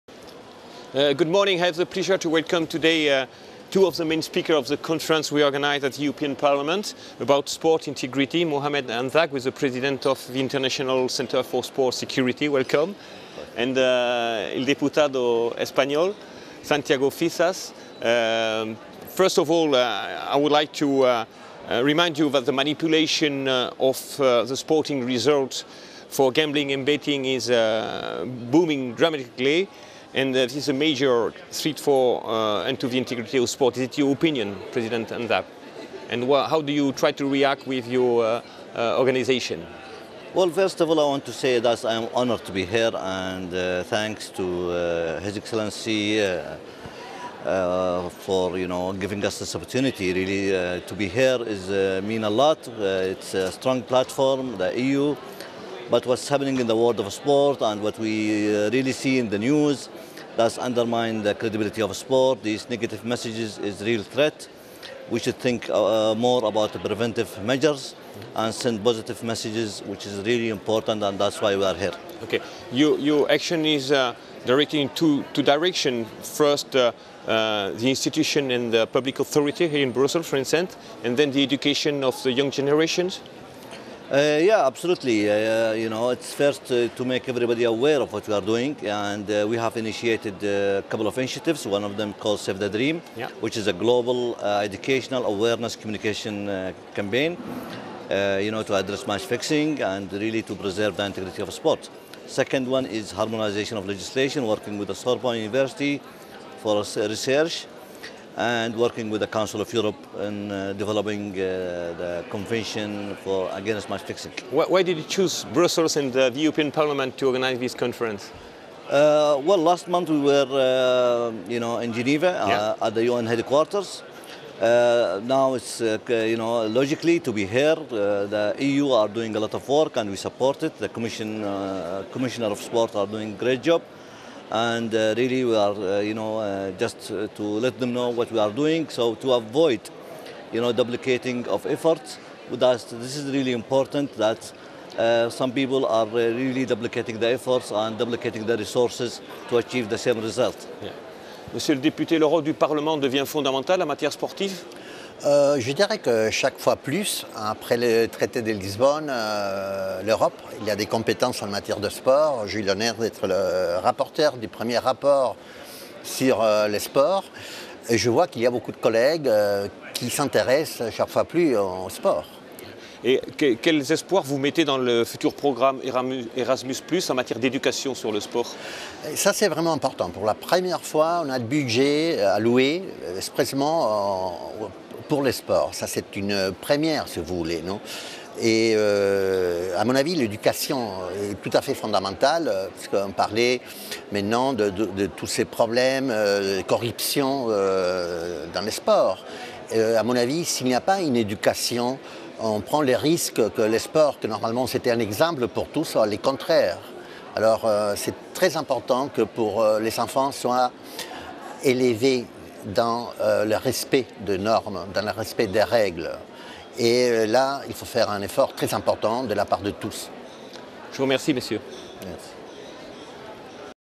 Sport Corner n°3 with Mohammed Hanzab et Santiago Fisas Ayxela
Sport Corners are debates recorded at the heart of the EU institutions, in the European Parliament in Brussels.

This unique show on sport and Europe is presented by Laurent Thieule (Sport and Citizenship President).

Guests :
Mohammed Hanzab,ICSS President
Santiago Fisas Ayxela, member of European Parliament